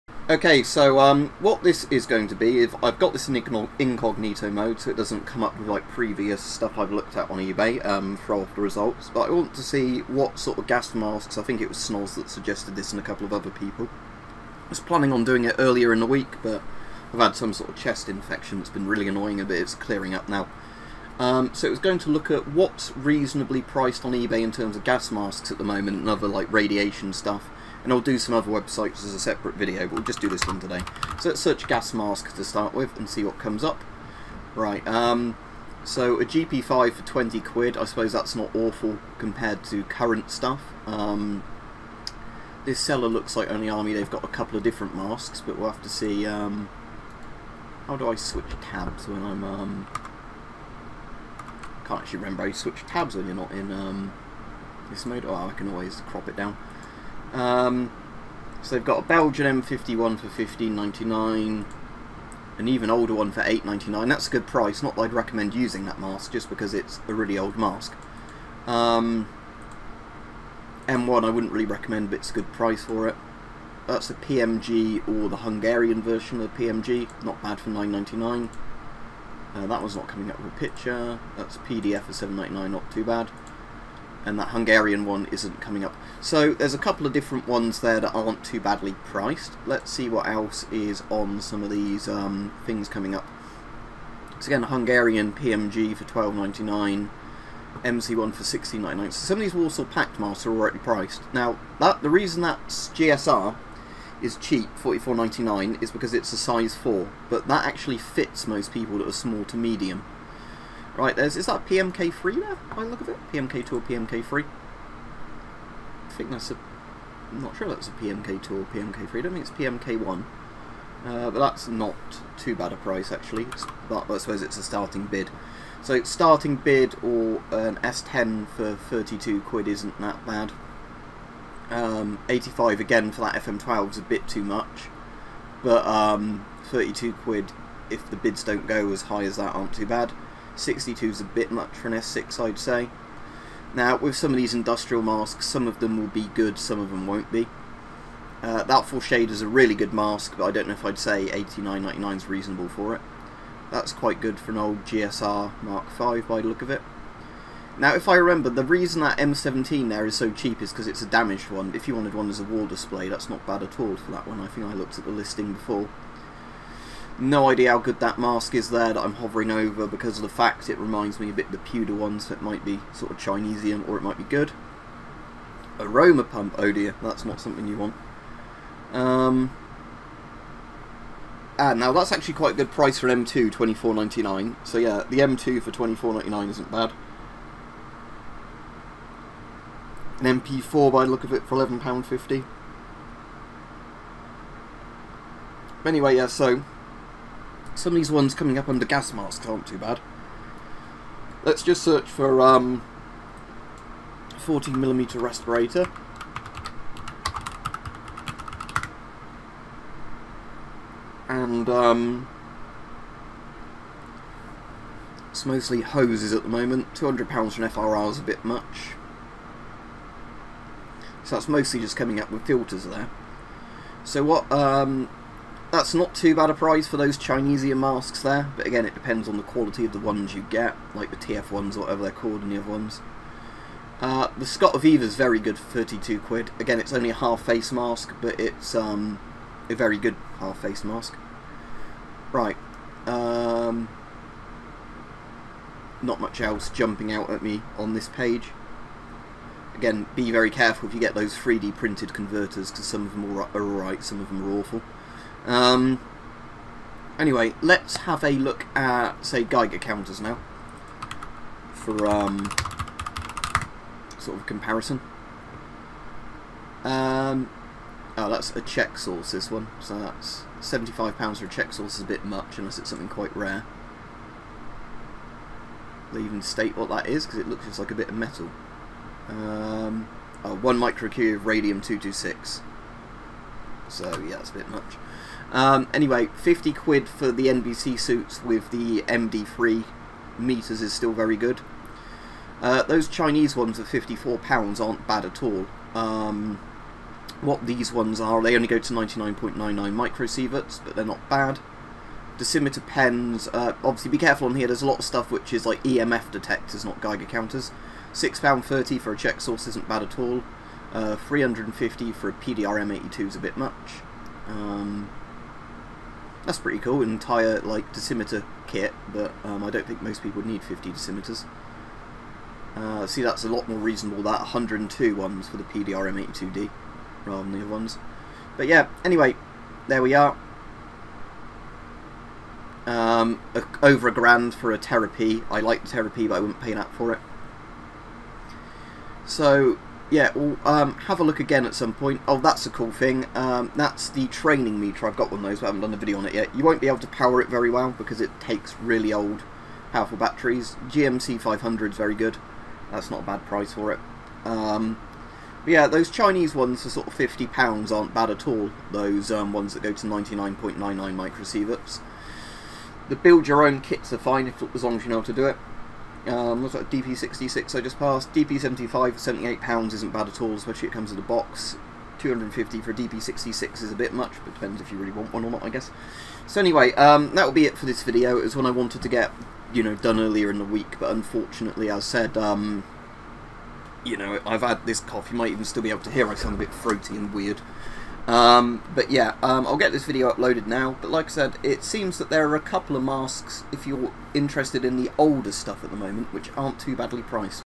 Checking eBay for decent CBRN prices
Just a quick video checking eBay for prices that seem reasonable compared to current prices for CBRN gear, there are a few decent masks and Geiger counters currently for sale.

I was planning on filming this video on Monday but ended up coming down with a chest infection so didn't feel well enough to do it to today, I will stream tonight 30/06/22 if I still feel okay.